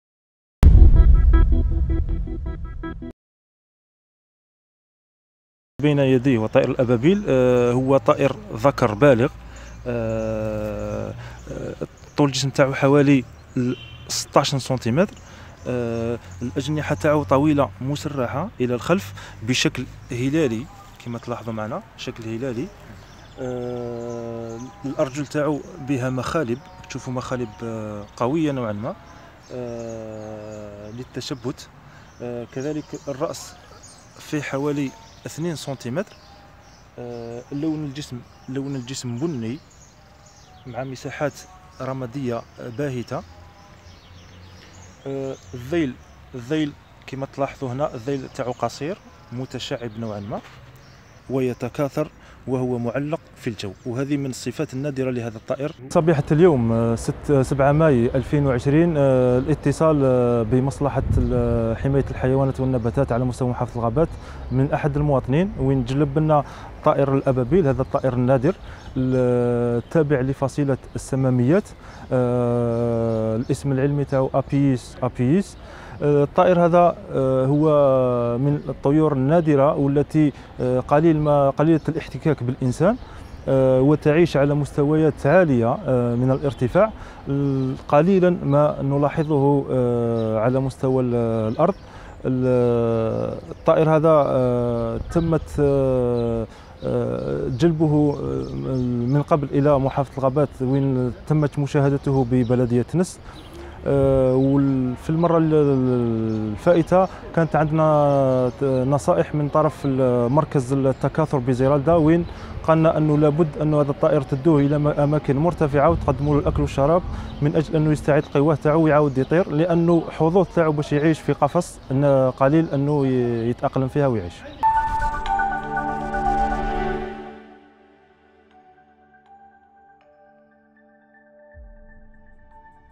الشلف: العثور على طائر الأبابيل النادر وتسلميه لمصالح الغابات
الشلف (وأج) - عثر أحد المواطنين بالشلف على طائر ينتمي لفصيلة السماميات أو ما يعرف بطيور الأبابيل، حيث قام بتسليمه لمحافظة الغابات التي عاينته ووضعته تحت الحماية.